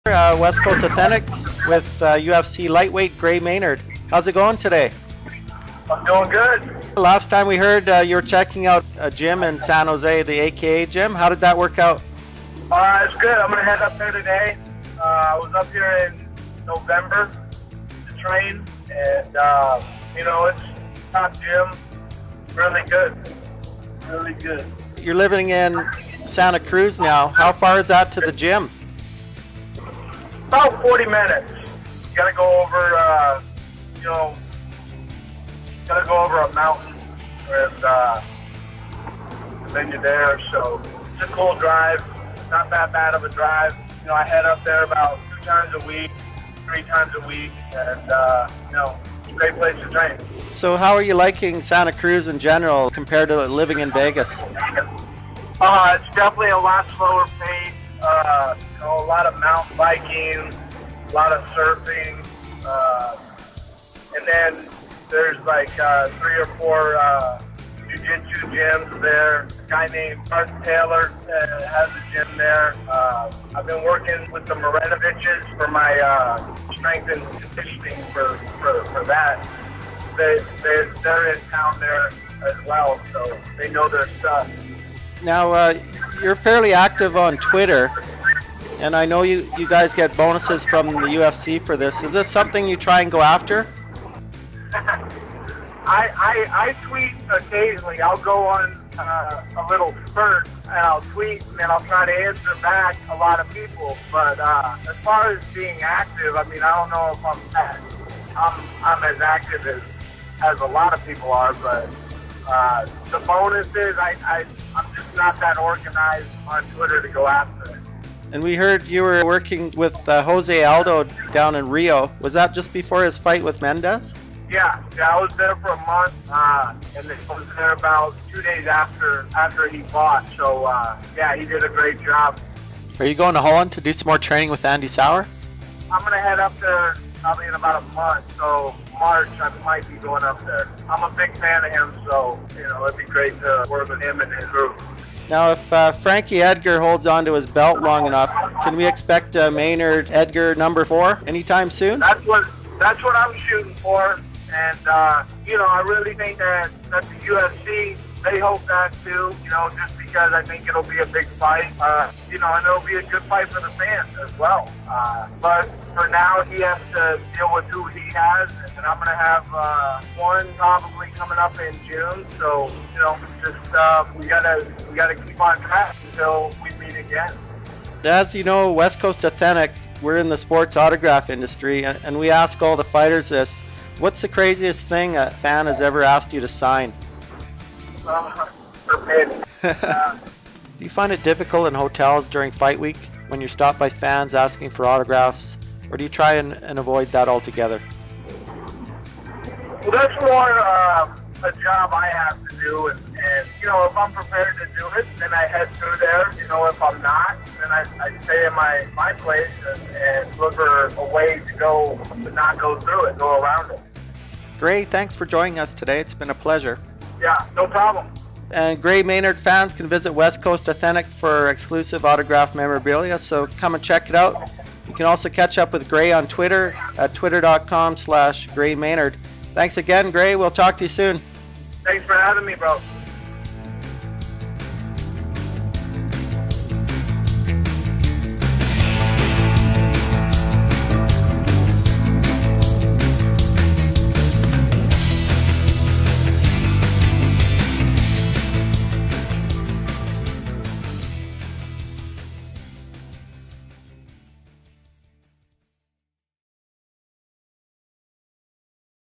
West Coast Authentic Interviews UFC's Gray Maynard
We had the pleasure of speaking to Gray Maynard about his new training home in California, training abroad with other fighters (Jose Aldo, Andy Souwer), anticipated Edgar-Maynard4, and the autograph market. Gray Maynard has an exclusive contract with West Coast Authentic for all autographed MMA/UFC memorabilia